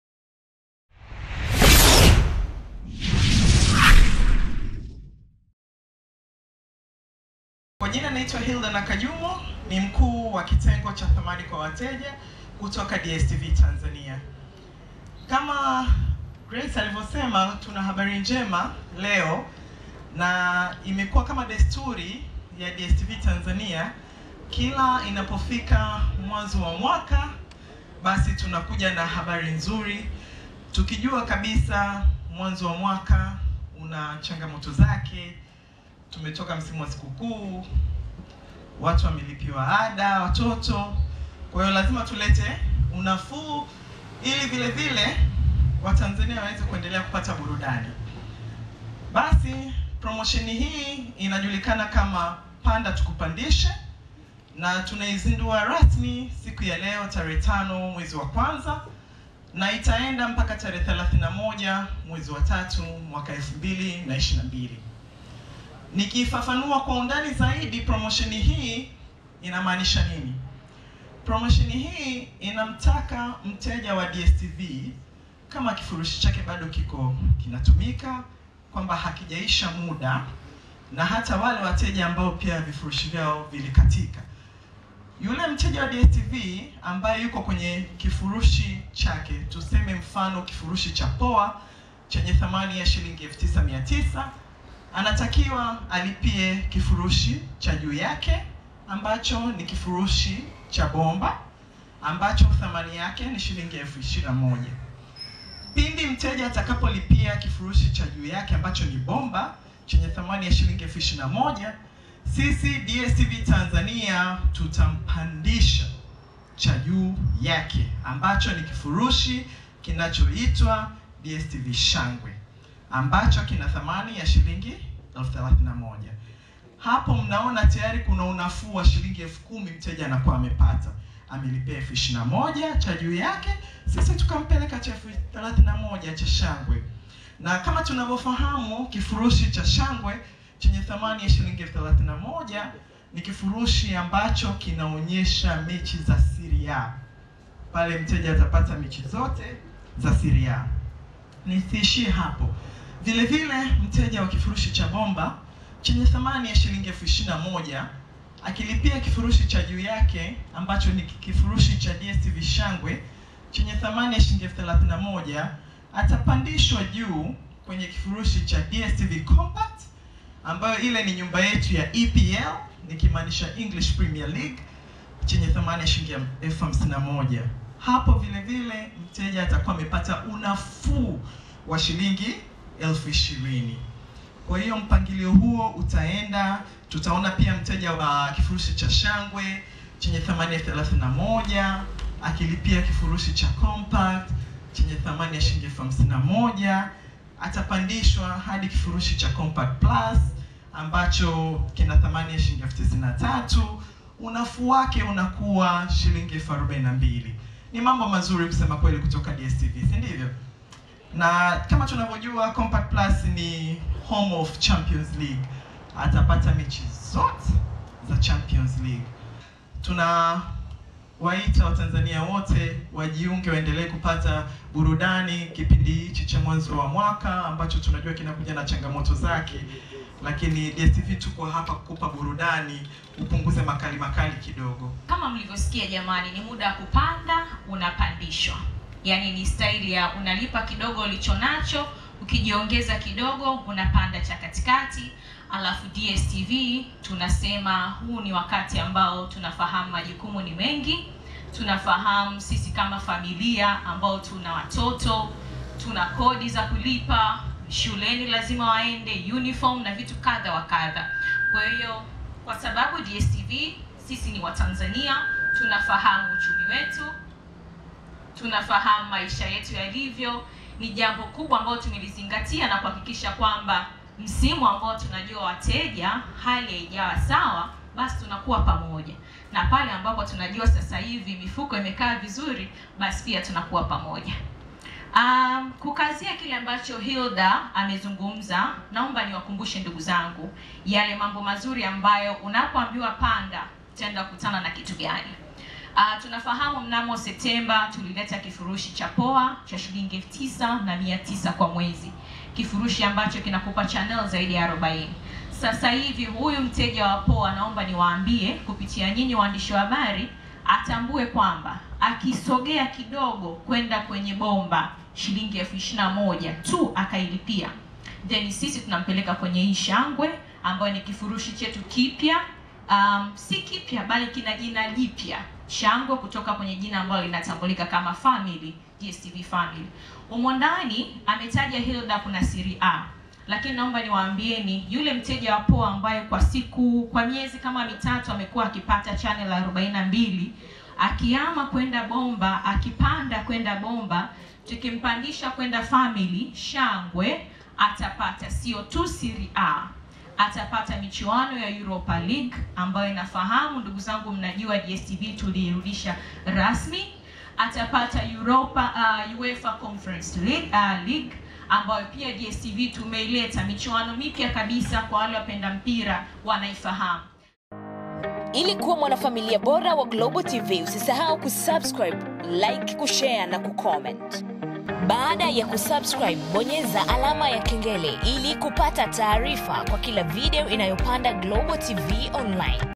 NOMA! DSTV Waja na 'PANDA TUKUPANDISHE' Maalum kwa KUPUNGUZA UGUMU wa MAISHA JANUARI HII..
Kampuni ya Malt Choice Tanzania kupitia King'amuzi chao cha DSTV leo wamezindua promosheni mpya inayokwenda kwa jina la Panda Tukupandishe ambayo ina lengo la kuwapunguzia maumivu ya Januari wateja wao.